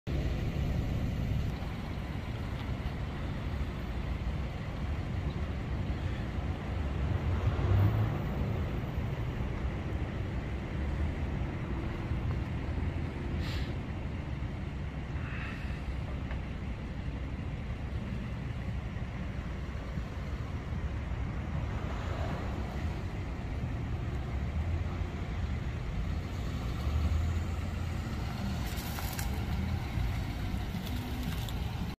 2013 Dodge Grand Caravan van for sale at auction | bidding closes February 22, 2022
2013 Dodge Grand Caravan van
Miles: 370,860 on odometer
VIN: 2C4RDGBG8DR506895
Engine
Displacement: 3.6L V6 DOHC 24V
Cylinders: 6
Fuel type: Gas
Transmission
Automatic
Interior
AC, Heat
Power windows, Power locks
Cruise control
Tires
Size: 225/60R16
Notes
It's located in Fort Smith, AR, and we're selling it to the highest bidder regardless of price.